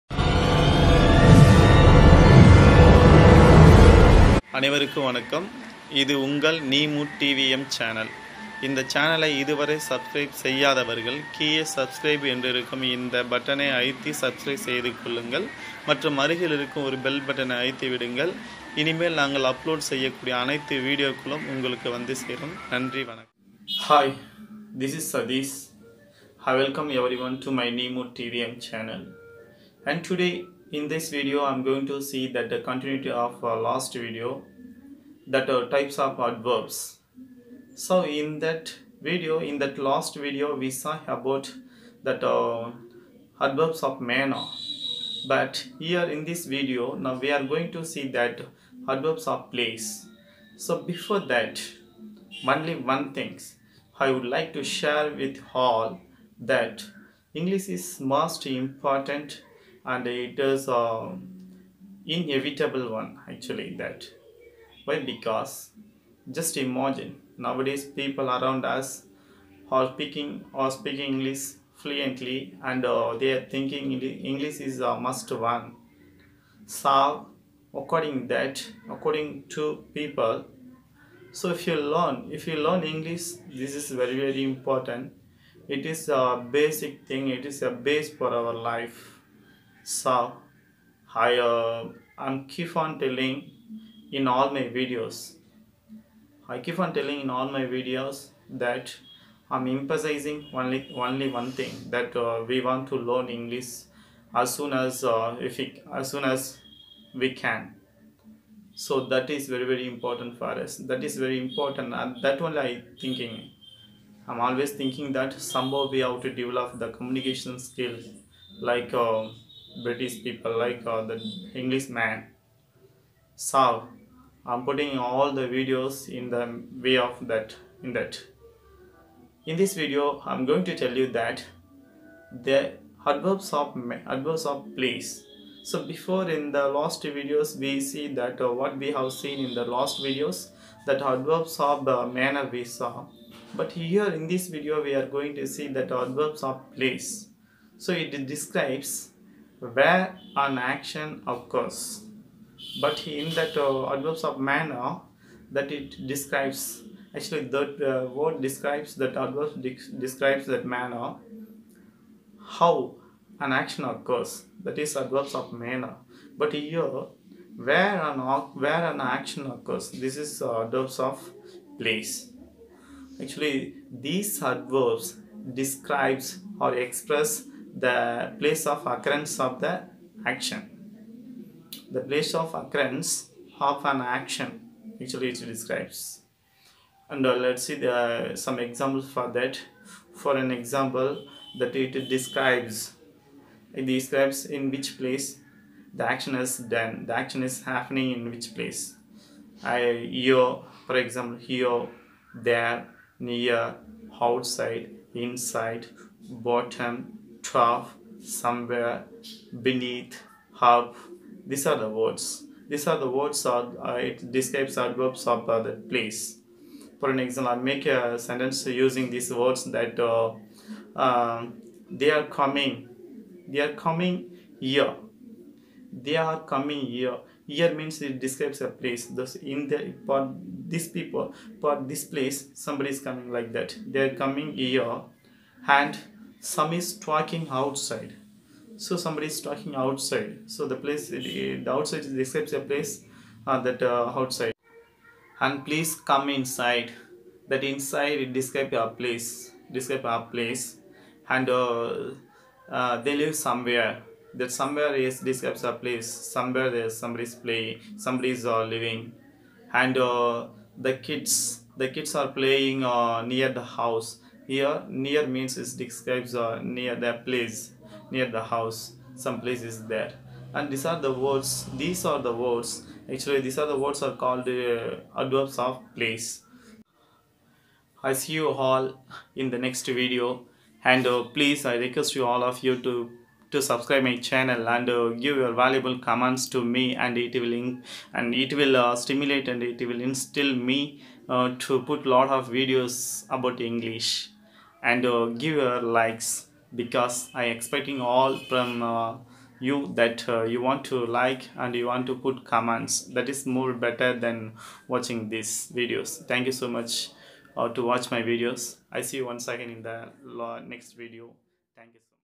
Adverbs of Place
I explained the Adverbs of Place with examples